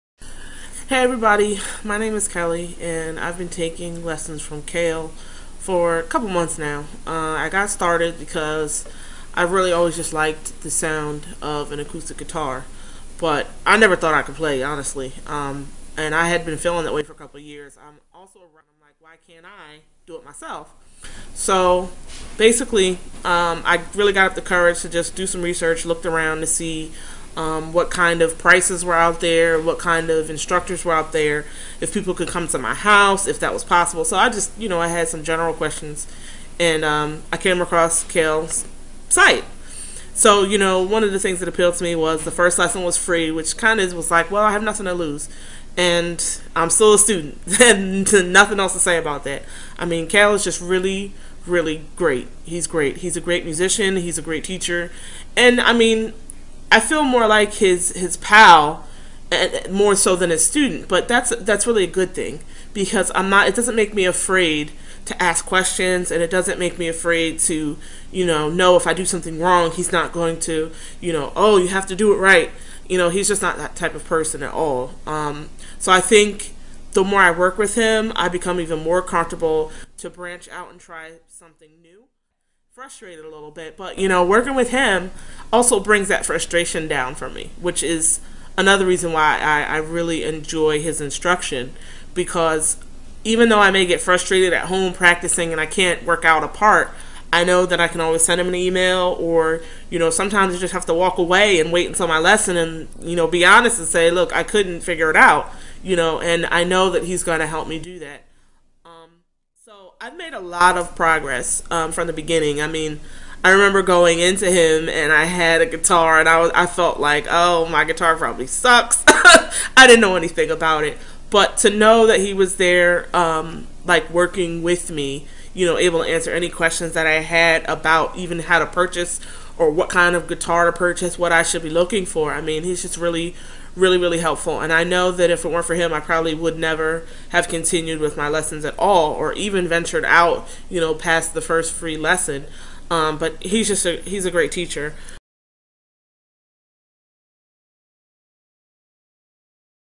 KGGS Student Feedback: Kelly Johnson
Kelly Johnson describes her experience as a student at Kale Good Guitar Studio.